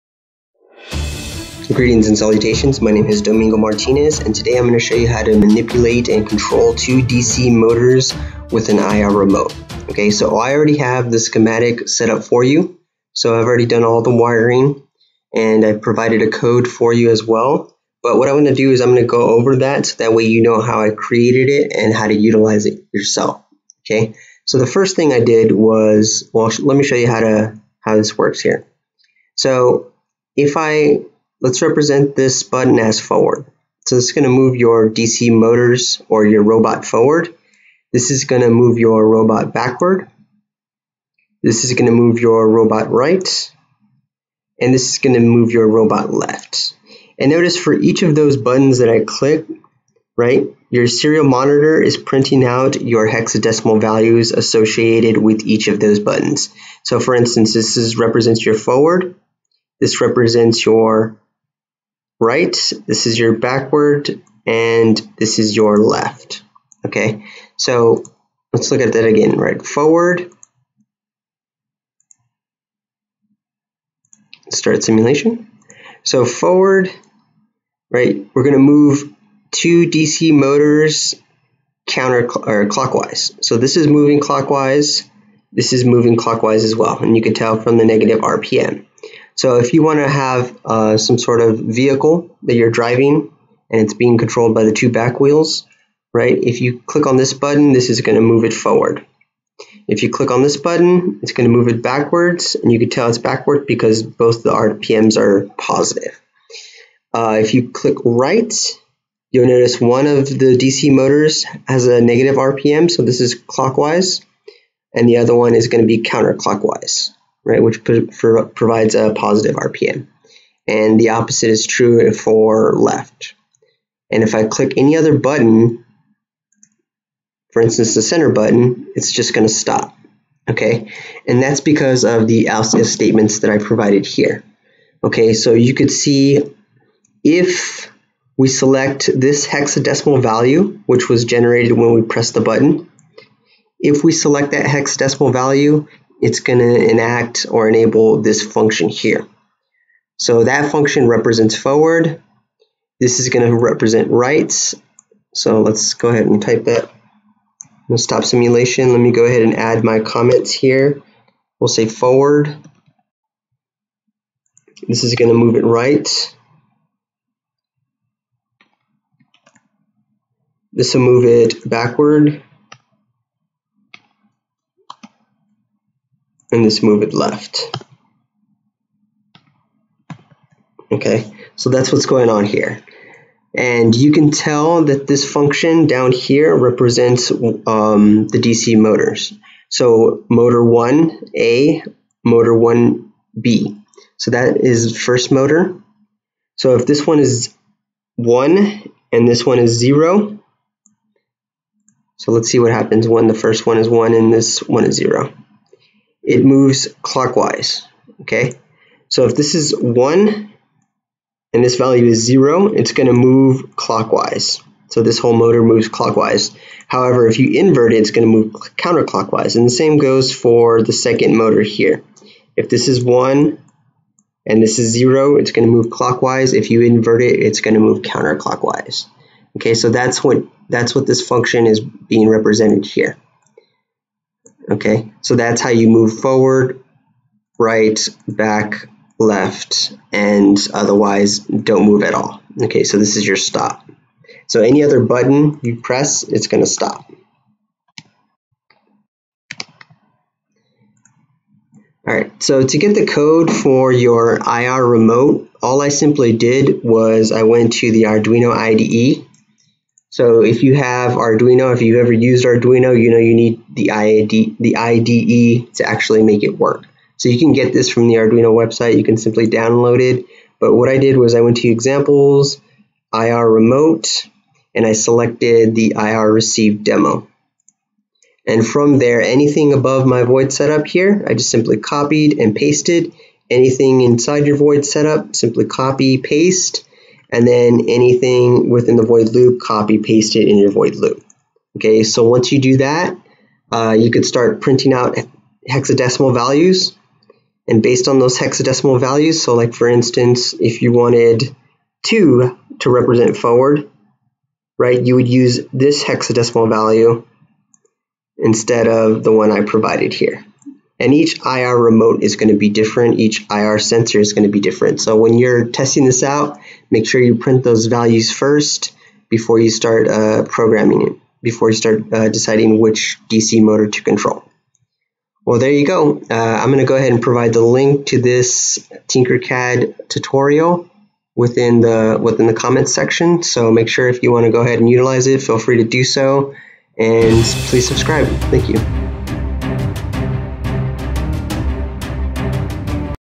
Using an IR Remote To Control DC Motors
Use an Arduino to control DC Motors with an IR Remote in TinkerCAD Circuits. In this video, you will learn how to drive a simple robot (two DC motors) using forward, back, left, and right with an IR remote.